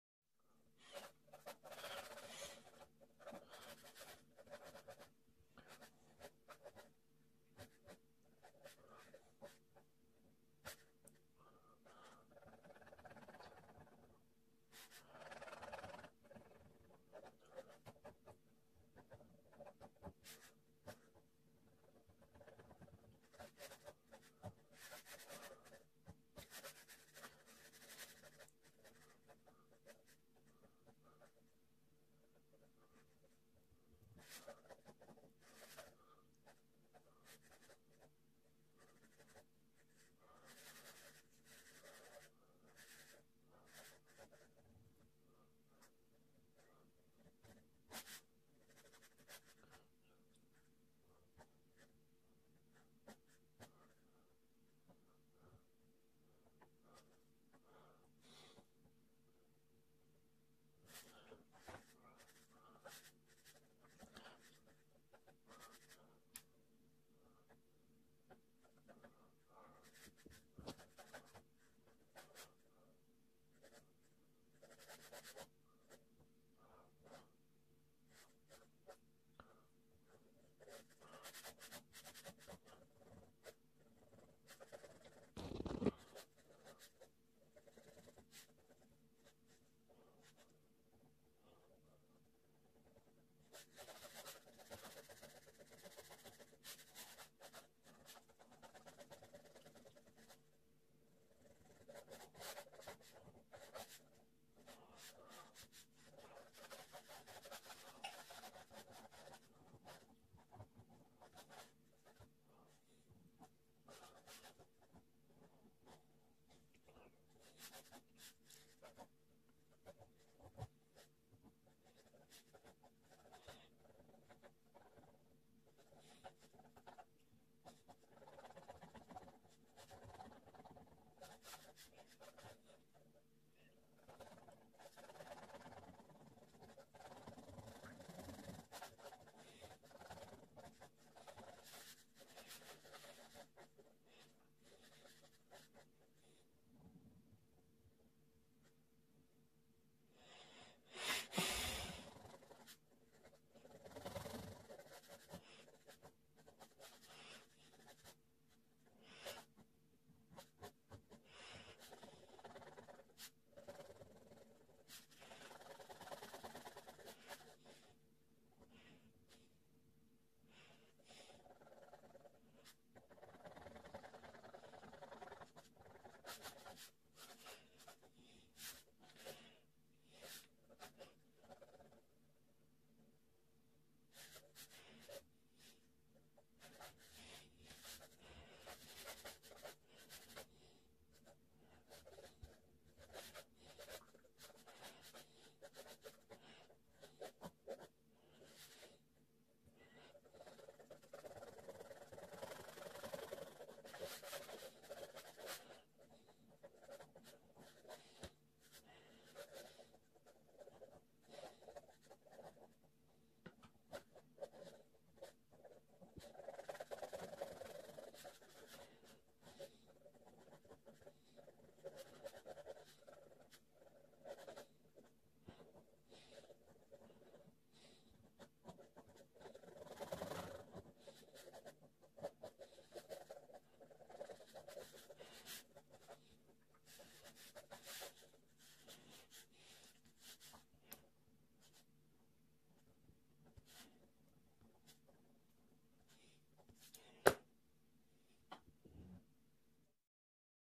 QUICK SKETCH by KEITH PROCTOR ARTIST
Topsy turvy Ted .. Sketch using a ballpoint pen.